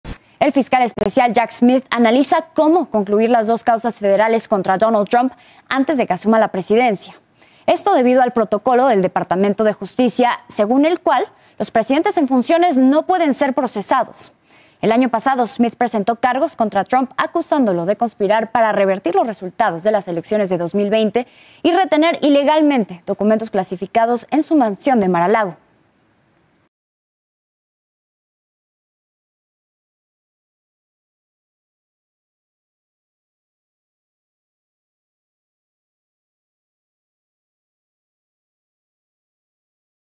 Analizan poner fin a acusaciones legales contra Donald Trump antes de asumir presidencia de EUA
El fiscal especial, Jack Smith, analiza cómo concluir las dos causas federales contra Donald Trump, antes de que asuma la presidencia de Estados Unidos.